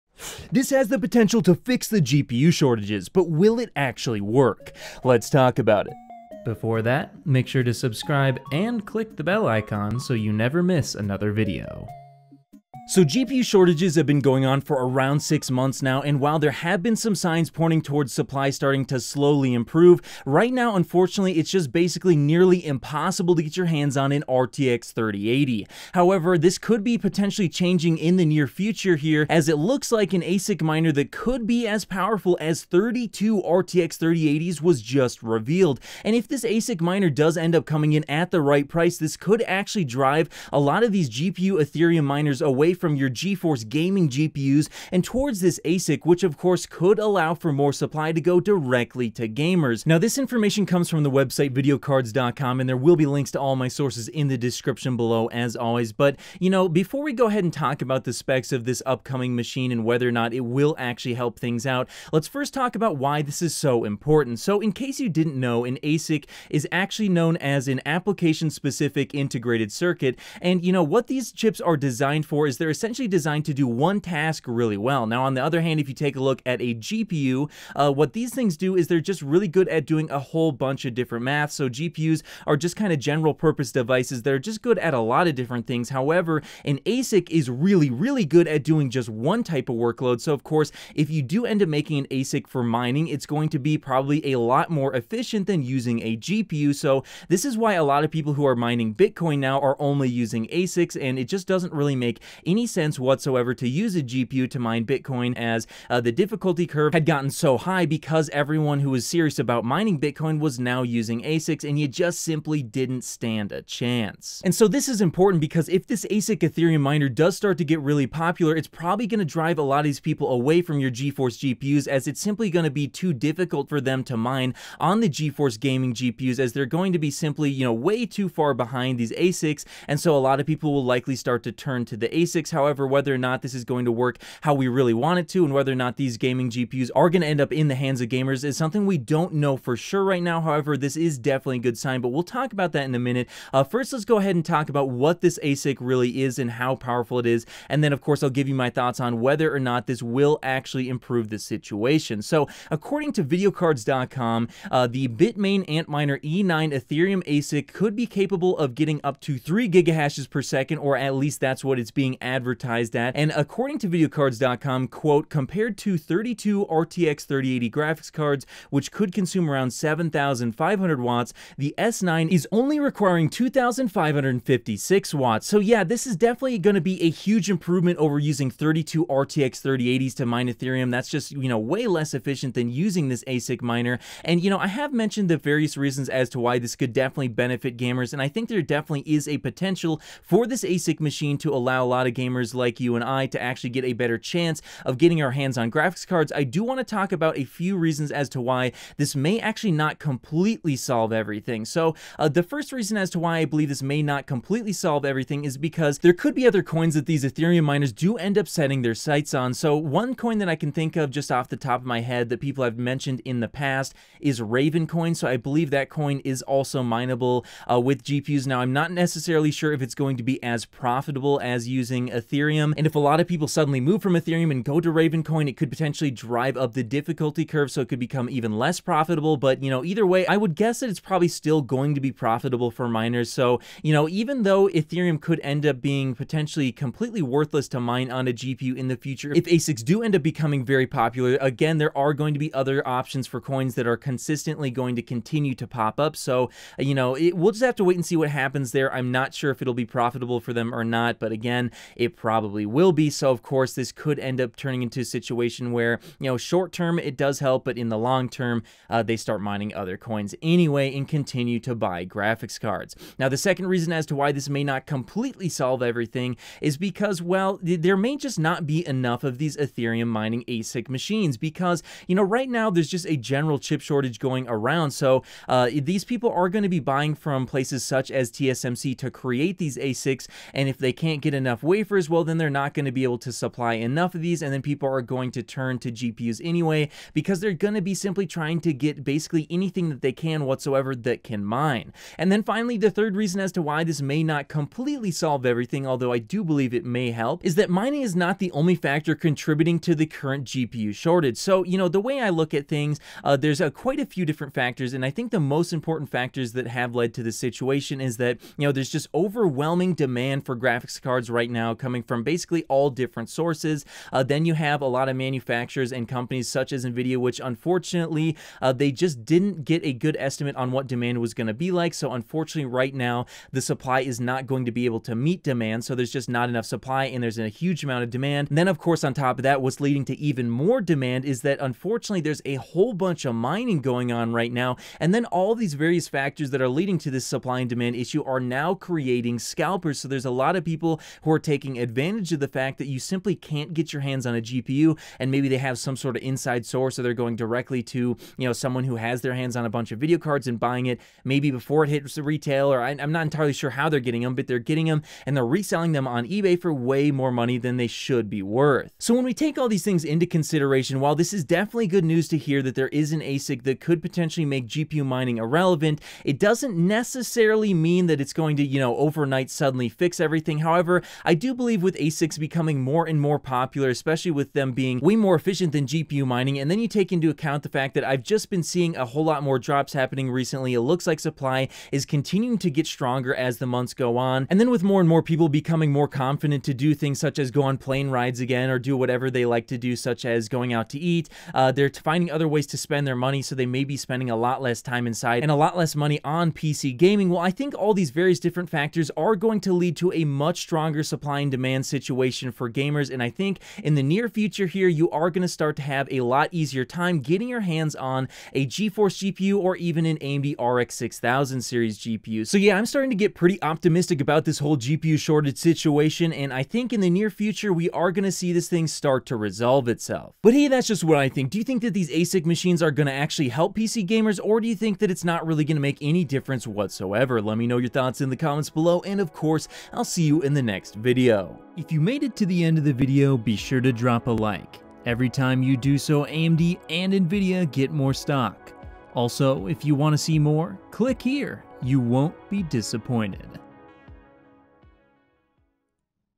This Could Be the END to GPU SHORTAGES
A new ASIC miner was just revealed recently that could finally put an end to mining on Nvidia GeForce RTX GPUs such as the RTX 3080 which is great news for PC gamers. The Bitmain Antminer E9 Ethereum ASIC miner is supposedly capable of up to 3 Gigahashes/s  and will be as powerful as 32 RTX 3080s. The question is will it be cheap enough.

FULL DISCLOSURE
END DISCLOSURE

Specs
CPU: R7 5800X
MOBO: ASUS X570 TUF Gaming WIFI
GPU: RTX 3080 FTW3 Ultra
CPU Cooler: NZXT Kraken X62
Storage: 2.5TB PCIE/SSD